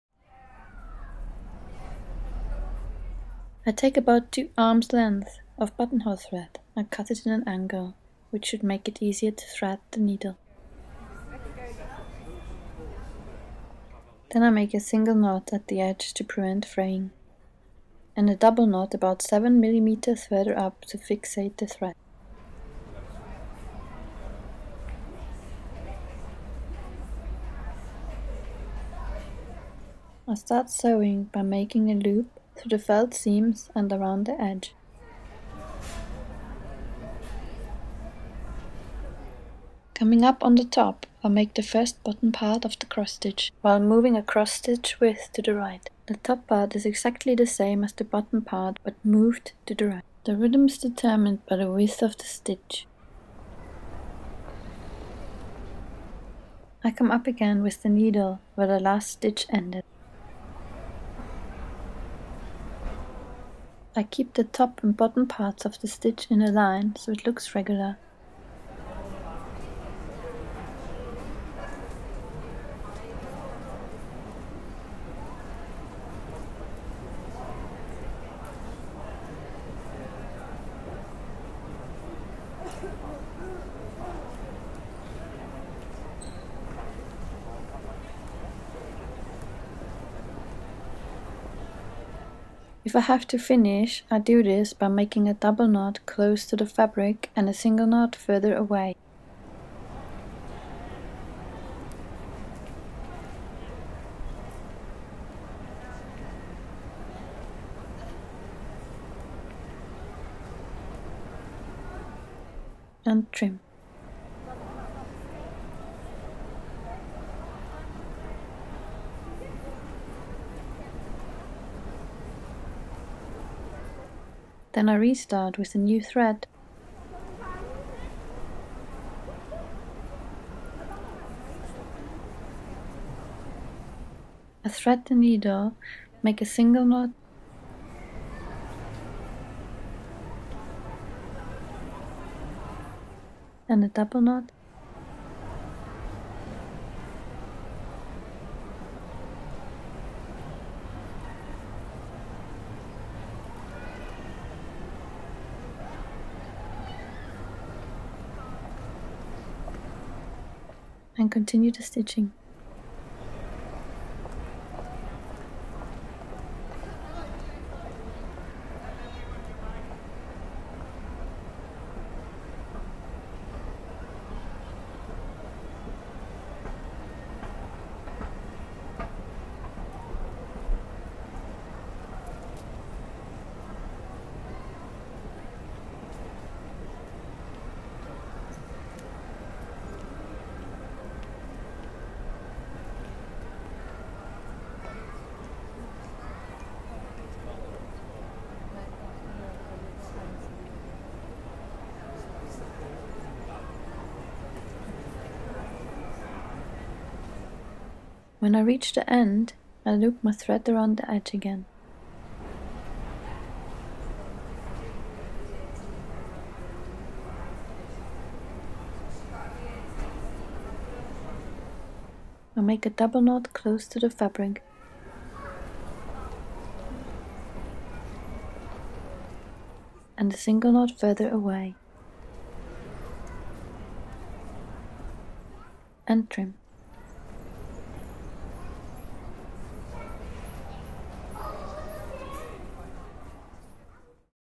the cross stitch - sewing by Hand
this movie shows how to sew a cross stitch (herringbone stitch) by hand, i can cu behind the scene

garments by i can cu